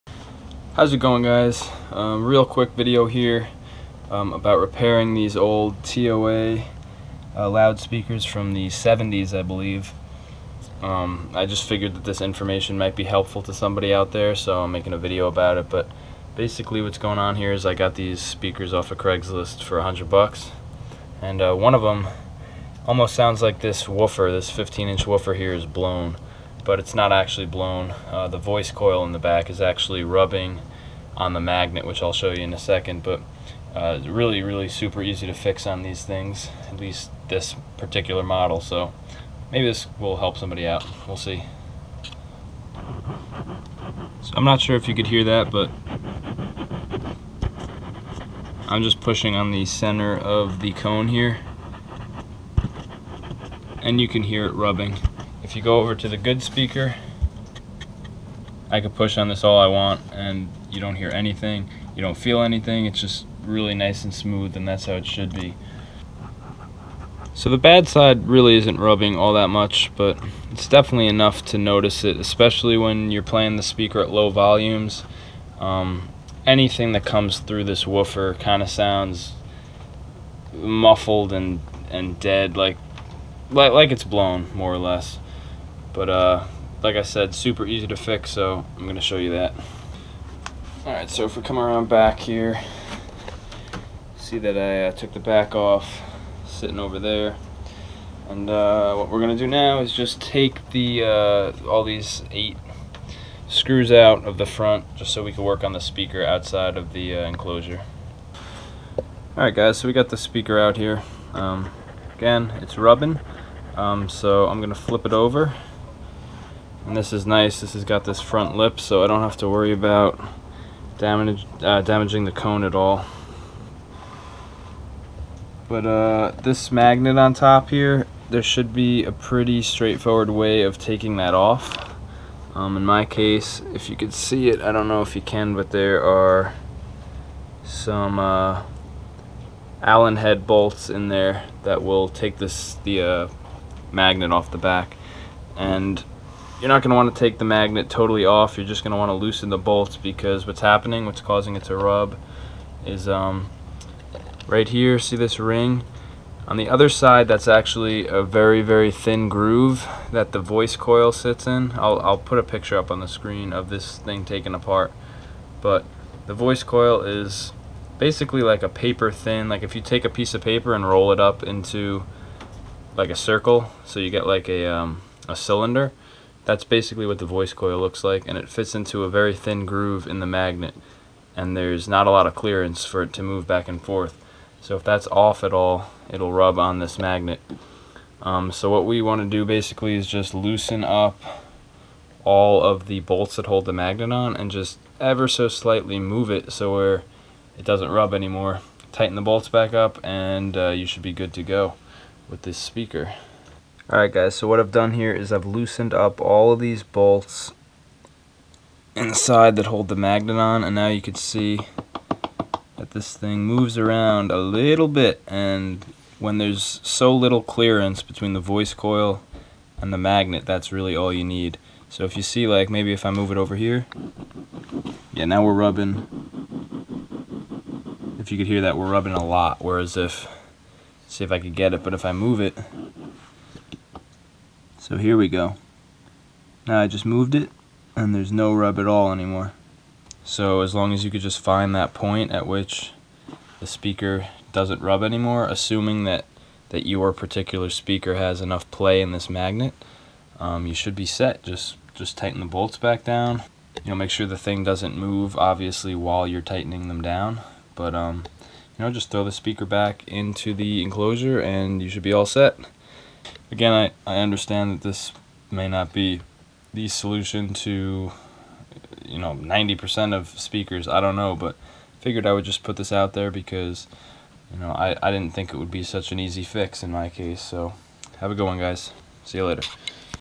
How to fix a 'blown' loudspeaker with coil rub
How to fix a speaker with coil rub, where the voice coil comes in contact with the magnet. This is often the case with speakers which have sat for extended periods (decades or more), causing the voice coil to droop into the magnet. In my case, the magnet had shifted after dropping the speaker and began to rub.